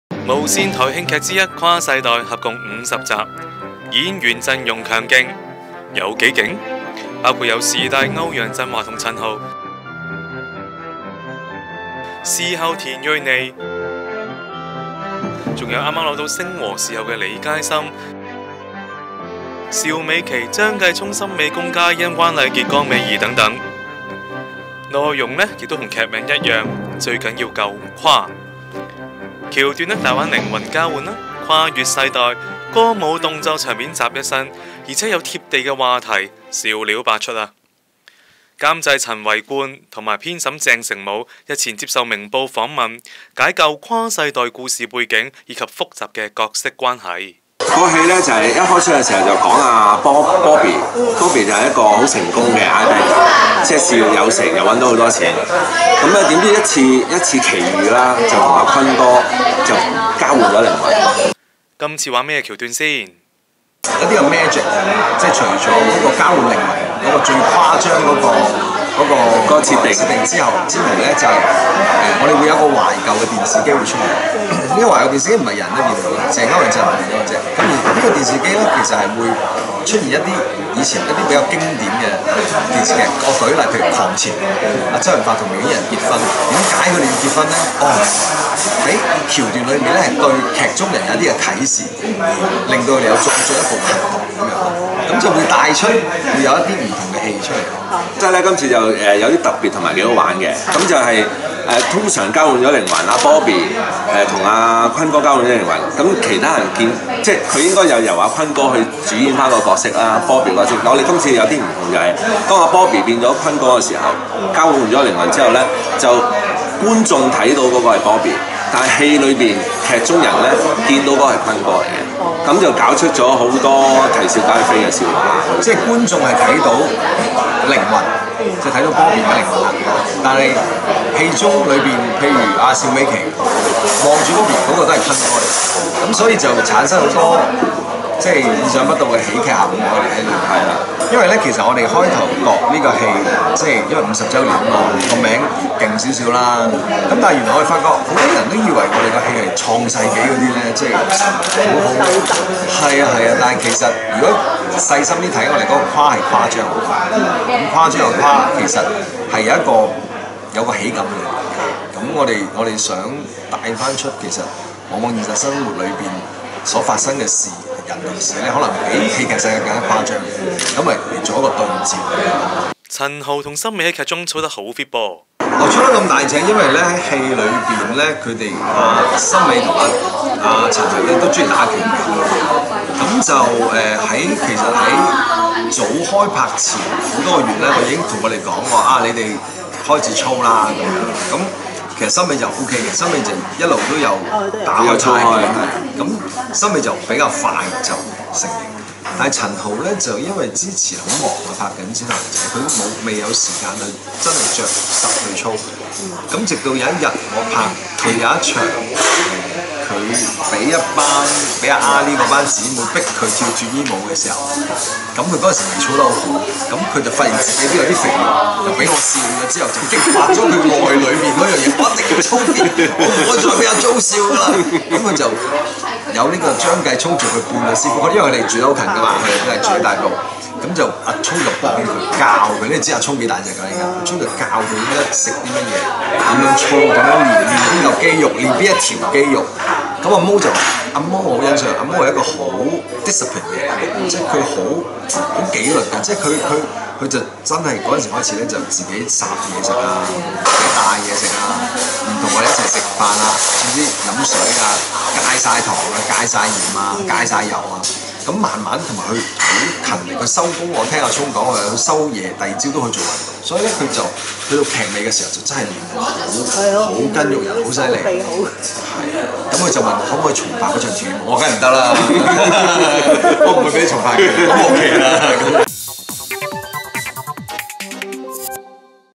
【最緊要「誇」】陣容強勁 監製陳維冠解構《誇世代》
無綫台慶劇之一《誇世代》合共50集，演員陣容強勁，包括視帝歐陽震華和陳豪﹑視后田蕊妮﹑星和視后李佳芯﹑邵美琪﹑張繼聰﹑森美﹑龔嘉欣﹑關禮傑和江美儀等。

劇集內容亦跟劇名一樣，最緊要「誇」，內容大玩靈魂交換﹑跨越世代，又有歌舞動作場面集一身，而且有貼地話題，笑料百出。監製陳維冠和編審鄭成武，日前接受《明報》訪問，解構《誇世代》故事背景，以及複雜的角色關係。